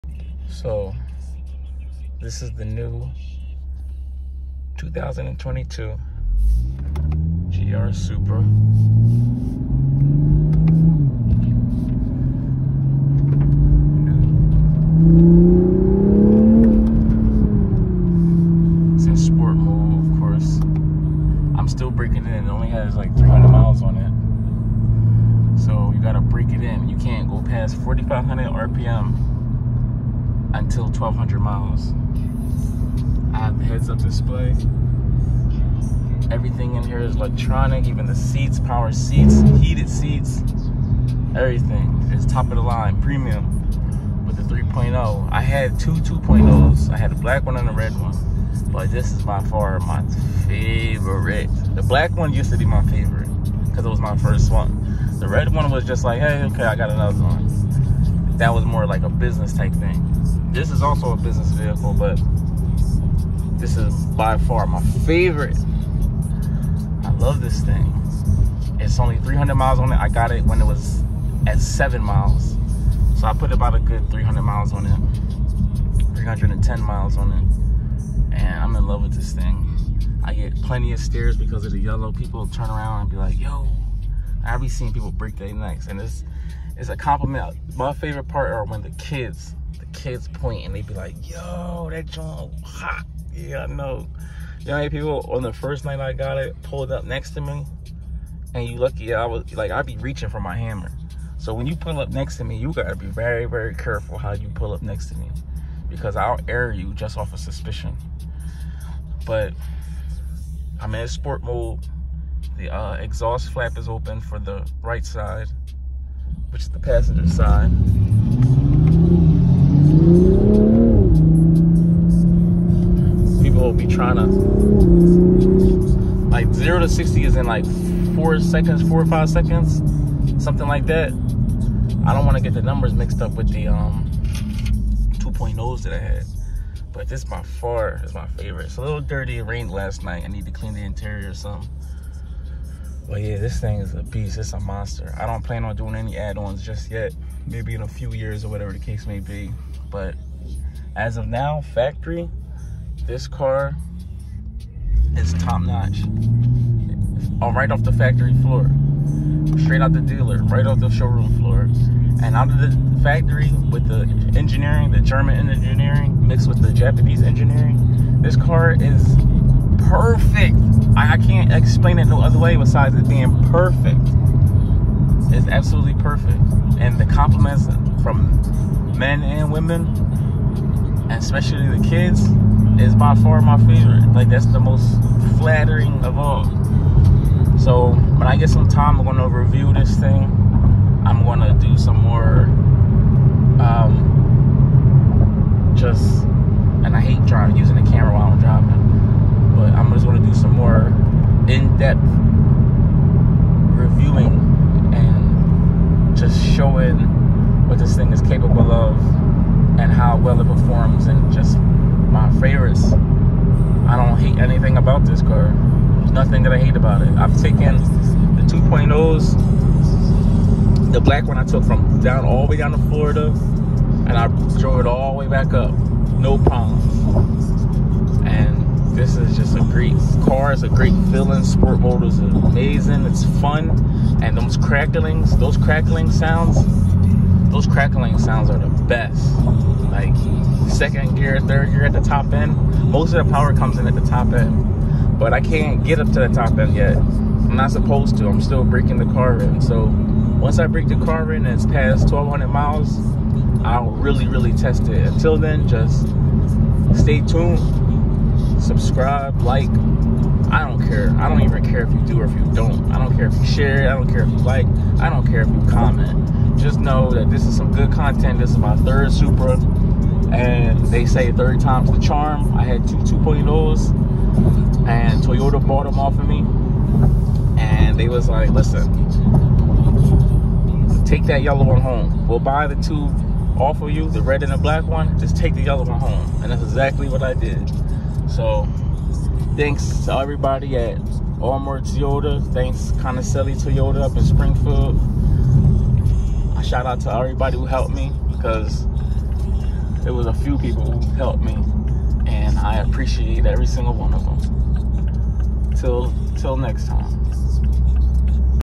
TRADED IN TWO 2021 2.0'S FOR THE 2022 GR SUPRA 3.0 PREMIUM (BEST DECISION EVER!)
The Hood’s First GR Supra Mane!